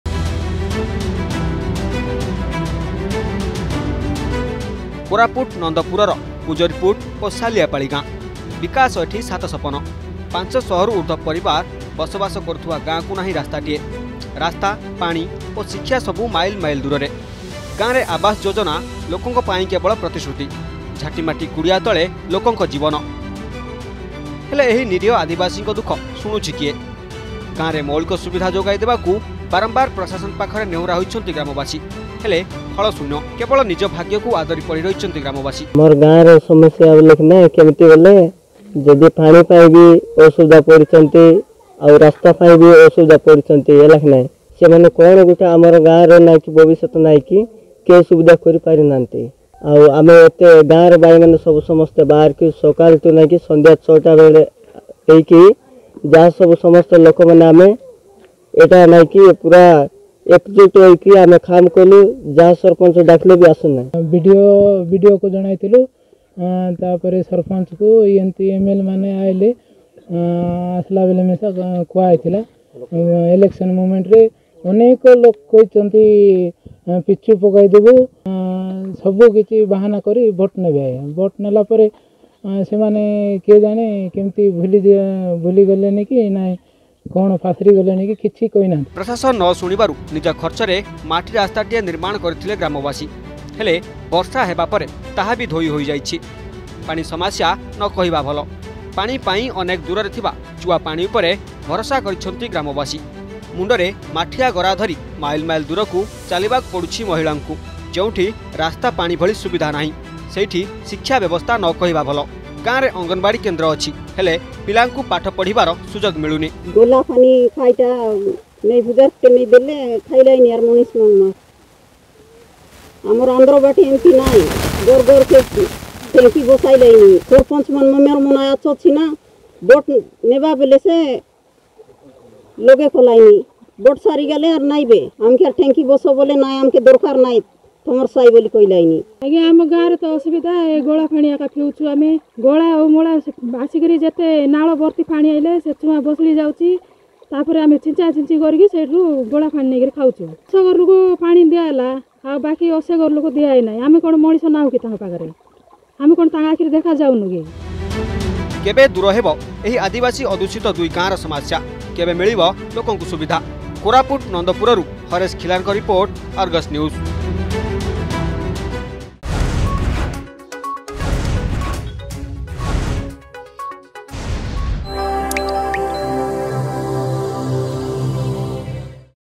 Numerous Govt Yojana to Develop Rural areas Yet Villagers are deprived of Basic amenities in Koraput
Koraput The villages of Nandapur Pujariput and Sariapal are far from being developed. Roads, safe drinking water, education, and healthcare are all unavailable to the tribal people that live here.